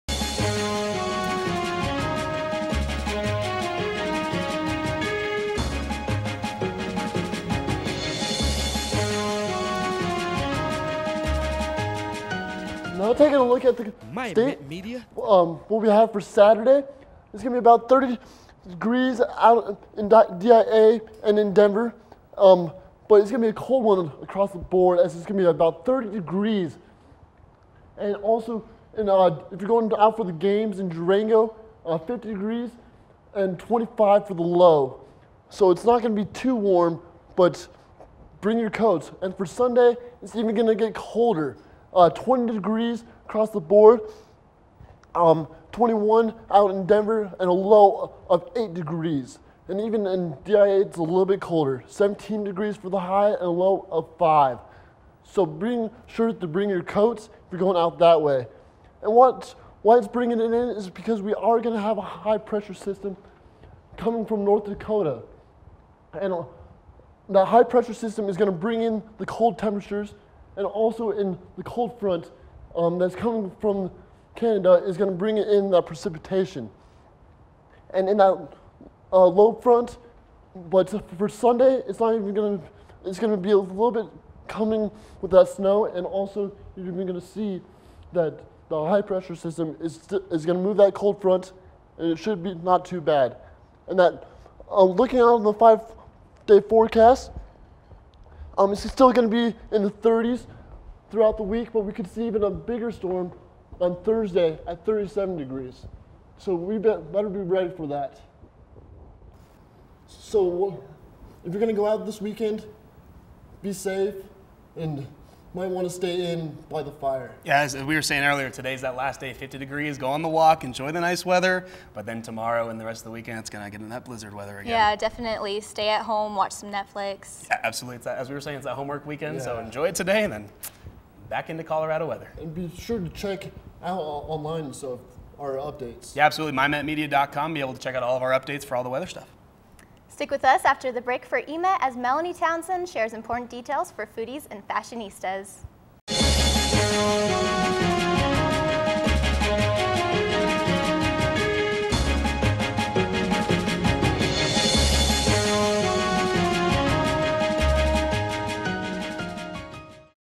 Weather 2.20.15
7-15" of snow could fall by the end of the weekend.  Foster Davey gives us the latest on the big February snowstorm.